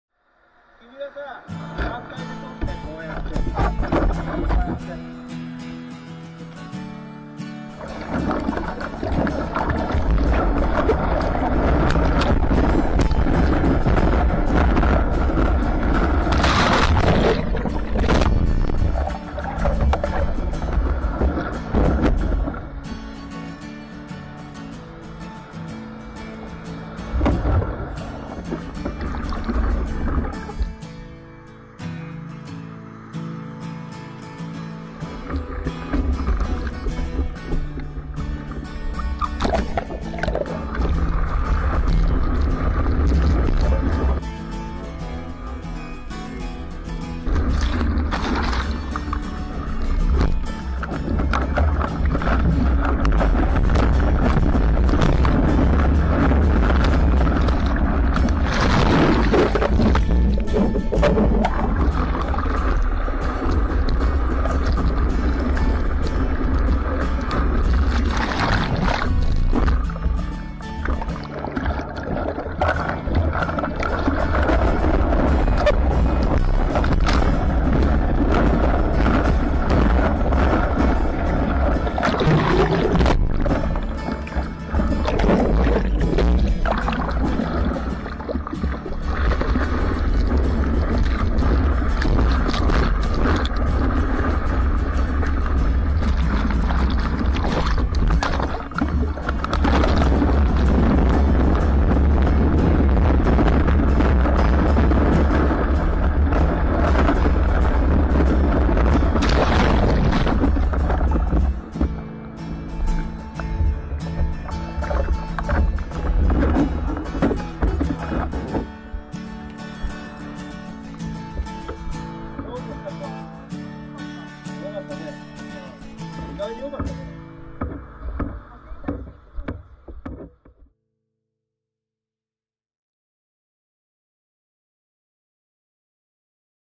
無風面ツルの小さな三角波の左右に2人で乗っていけた日本海のポイント 231030 ~サーフモンキーTV
10/30の午前10時頃の日本海は、無風でほぼ面ツルの小さな三角波がブレイク。2人でその左右に同時に乗れたこともありました。なかなかこんな良いコンディションはなくて、まさに2人のパラダイスでした。よろしければどうぞ！